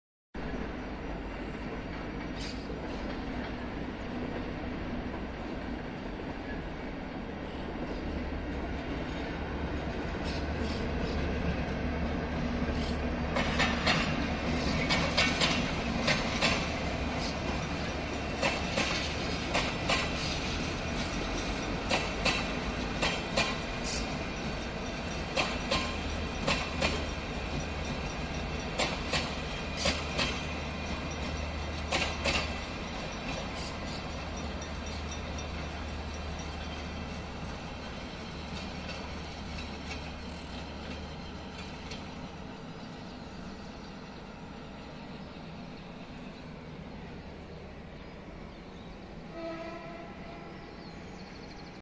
TA- Local a Córdoba saliendo de Villa María
Tren local de Trenes Argentinos a cargo de la locomotora EMD J26 CW/AC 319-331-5 saliendo de Villa María rumbo a Córdoba Capital. 28/01 07:03 AM.